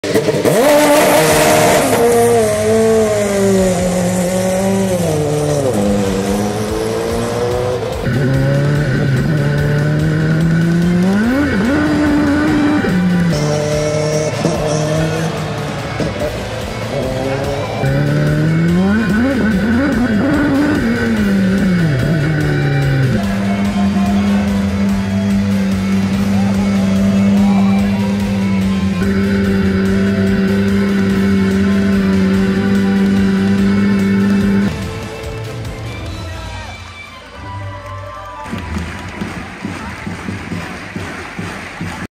スタジアムを激走 │ スーパーフォーミュラ
川崎フロンターレの本拠地 等々力陸上競技場をスーパーフォーミュラ SF23 で激走！
ドライバー 関口雄飛 選手

Super Formula SF23 accelerates at full throttle at Todoroki Athletics Stadium, home of Japan's top soccer team Kawasaki Frontale!

SUPER FORMULA全戦ライブ&チーム無線が聴ける！
▷新デジタルプラットフォーム